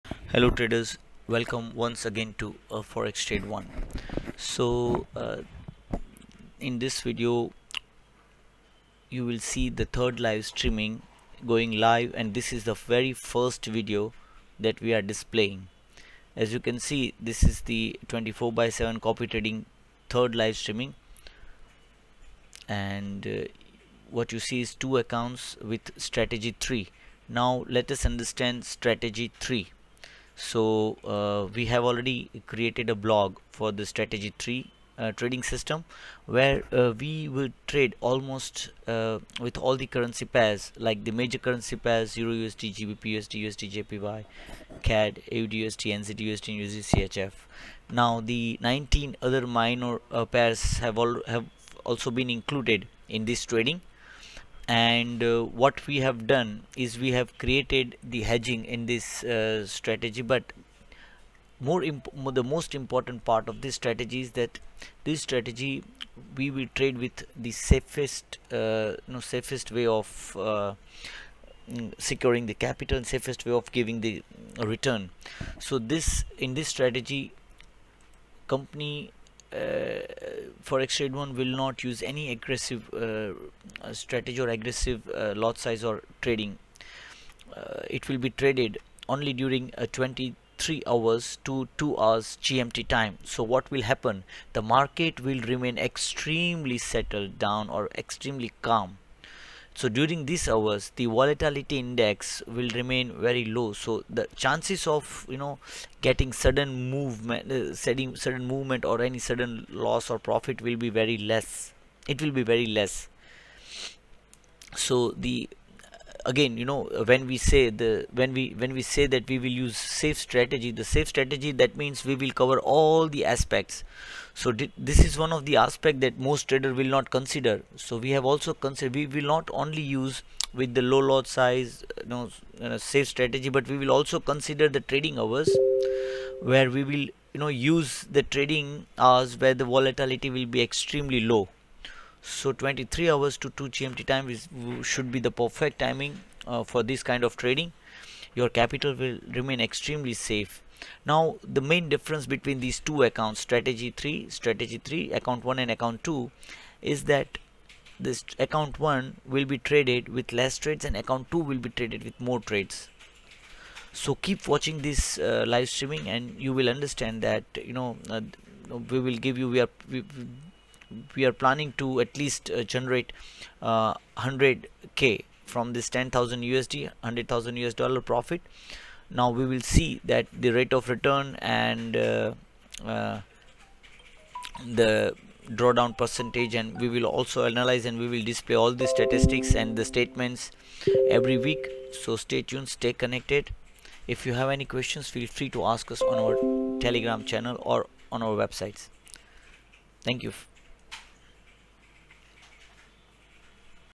Strategy 3 24x7copytrading expalined
2 Account Started
Strategy 3 Applied
Click here to know more about
« Strategy 3 »

Account  1 = $10k CAPITAL
Profit = $2000

Account 2 = $10k CAPITAL
Profit = $2500

» Safest Return
» Total Trading Time: 3 hrs
» Trade only placed in Cold Hours
» Trading Time 23:00 to 02:00 GMT
» You can verify live trades for 3 hours and can trade manually as SL & TP will be provided.
» Expected ROI 10% to 30% a Month
» Investor's First Choice = low-risk safe return